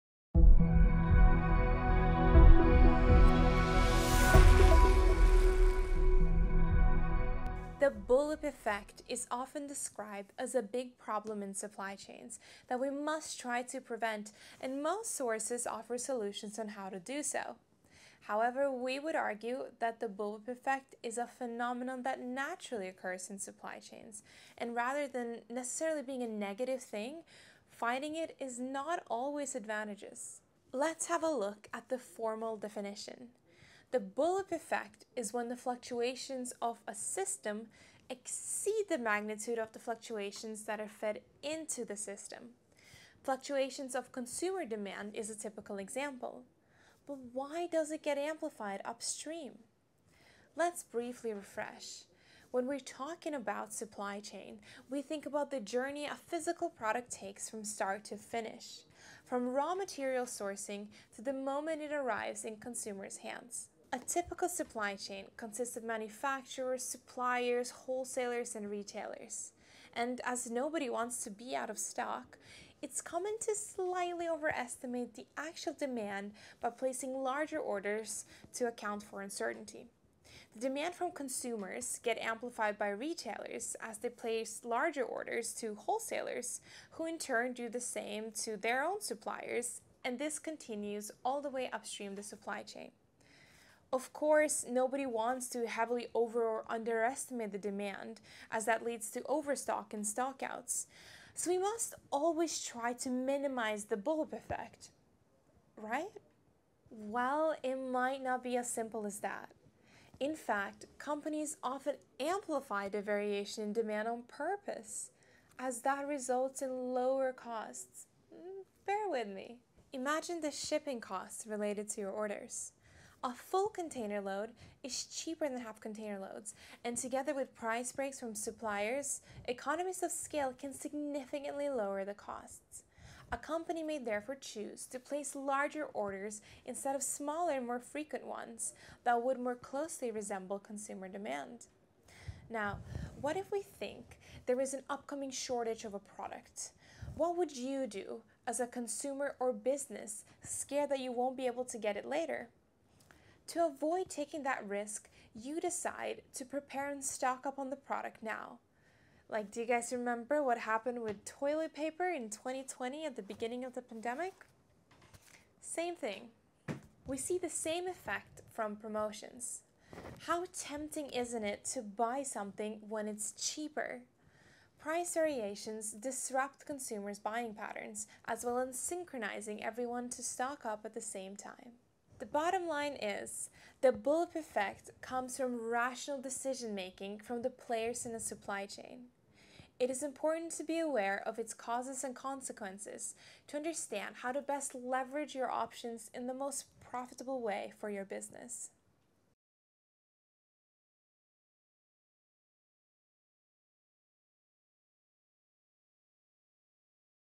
The Bullwhip Effect - Supply Chain in 3 Minutes
The Bullwhip Effect is a well known supply chain distortion, commonly described as a problem that must be avoided. At Lokad, however, we would argue that the bullwhip effect is a phenomenon that naturally occurs in supply chains, and minimizing it is not necessarily advantageous for your business.

Even thought nobody wants to heavily over- or underestimate demand, companies tend to amplify demand variations on purpose. A full container load is cheaper than a half container load, and together with price breaks from suppliers, economies of scale can significantly lower the costs. Furthermore, promotions attract customers from competitors but simultaneously disrupt consumer’s buying patterns as well as synchronizing everyone to stock up at the same time.

In conclusion, the bullwhip effect comes from rational decision making from the players in a supply chain. It is important to be aware of its causes and consequences to understand how to best leverage your options in the most profitable way for your business.